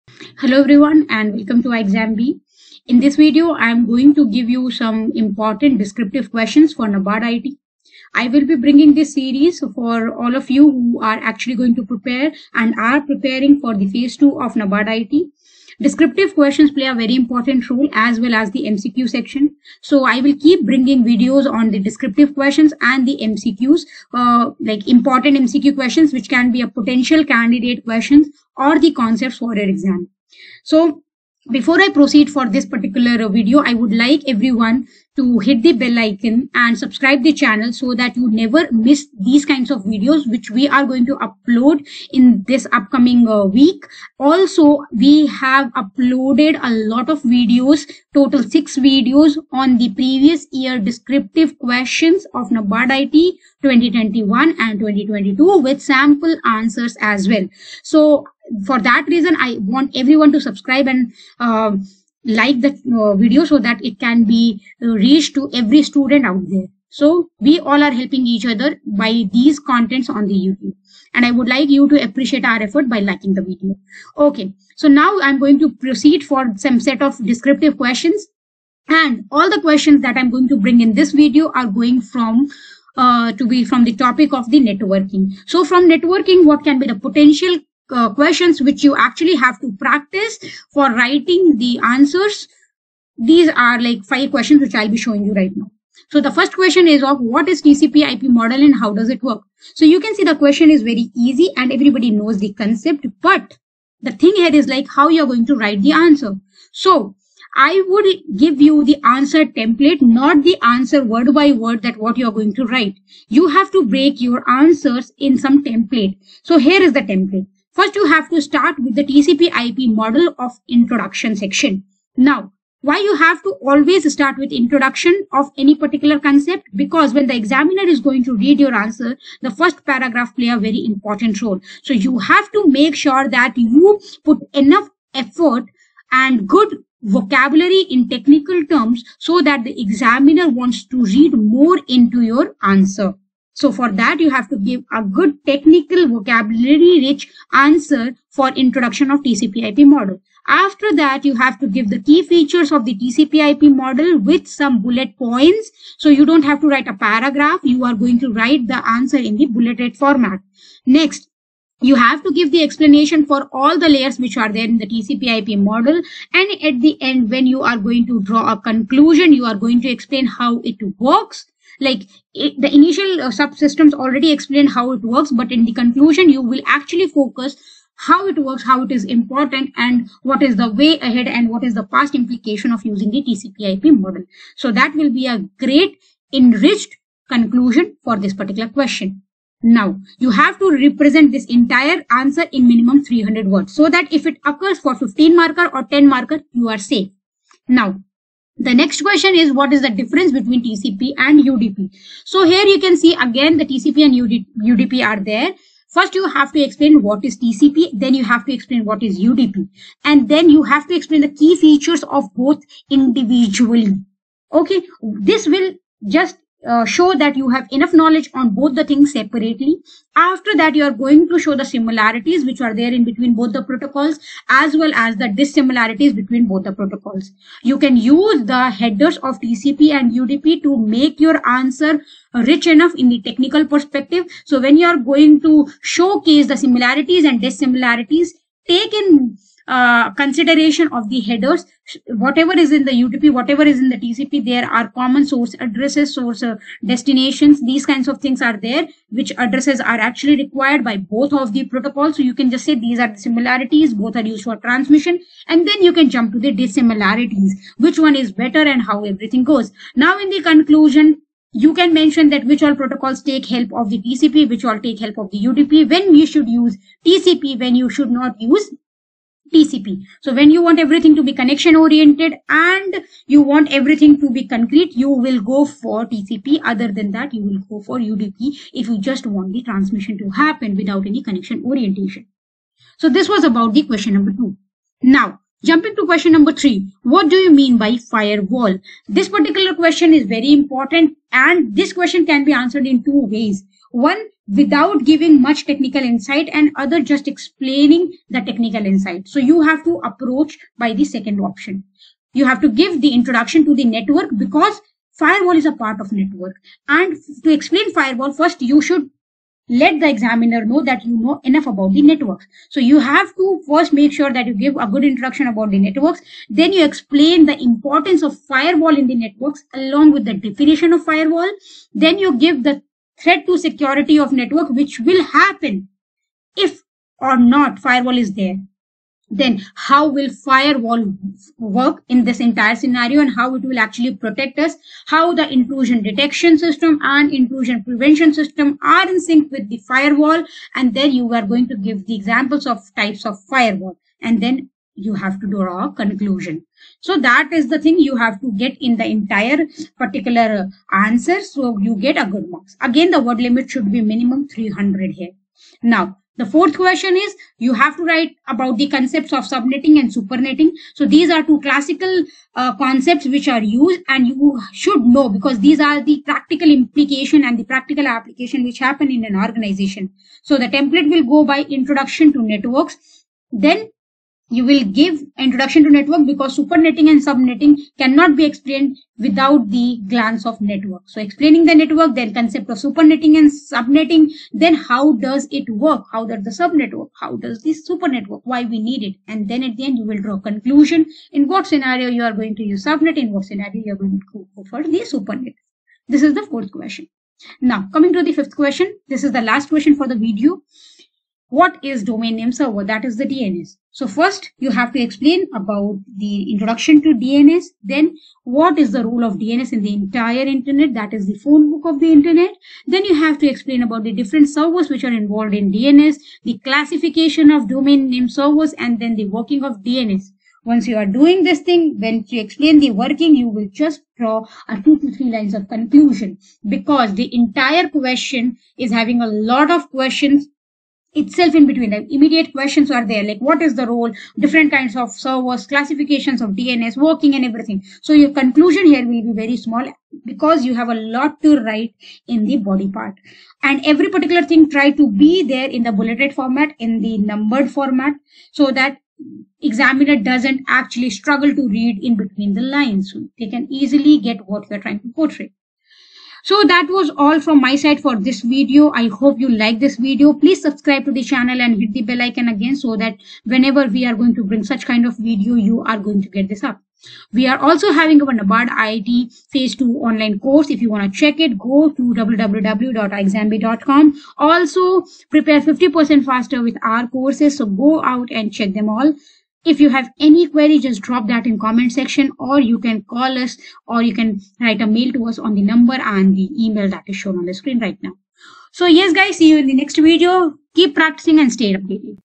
NABARD IT Recruitment 2023 | Important Descriptive Questions for NABARD IT Phase 2 | By Jayanti Mam
Hello Everyone, In this video Jayanti Mam has discussed the Important Descriptive Questions for the NABARD IT Exam. Watch this video till the end.

Request a call back for any query related to our best Online Courses, fill out this form
we will get back to you soon